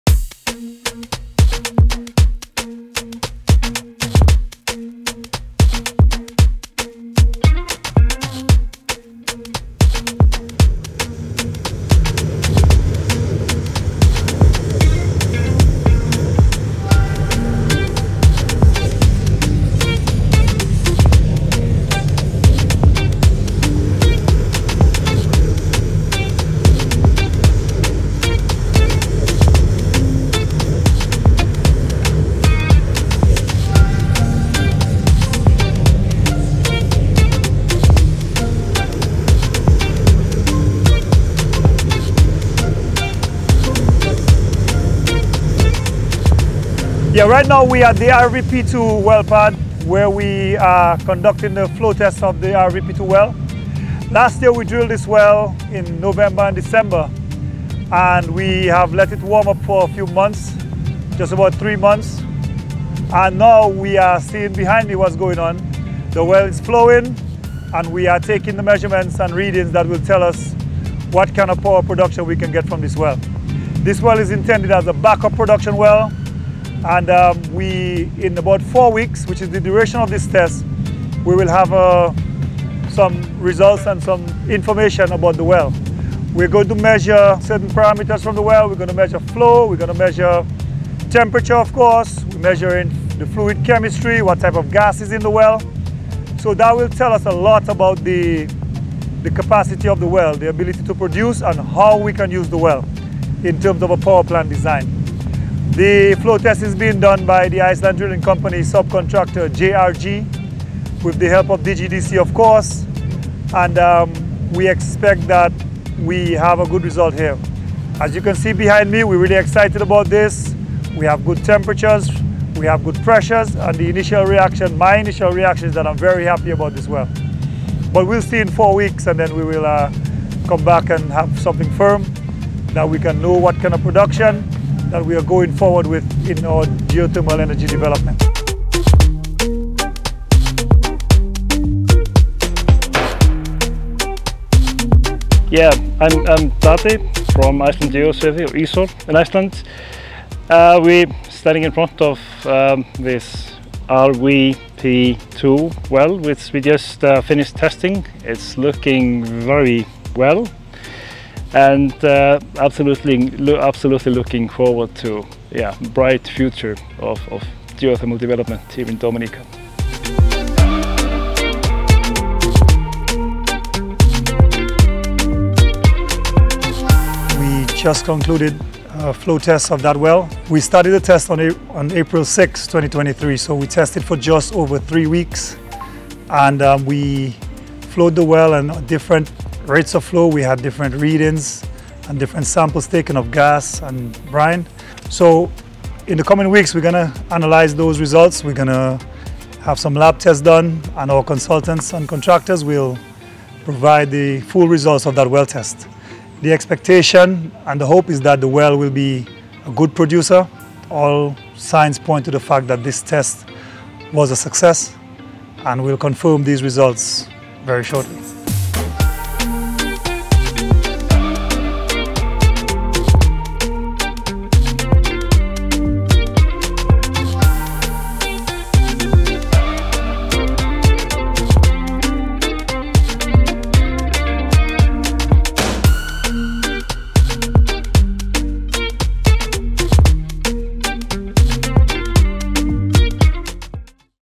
RV-P2 Production Well Flow Test
Dominica Geothermal Development Company Ltd (DGDC) has completed flow testing of the RV-P2 geothermal production well. The flow testing is done to determine the well's ability to generate energy. The DGDC would like to thank the Laudat community for cooperating with them during the testing.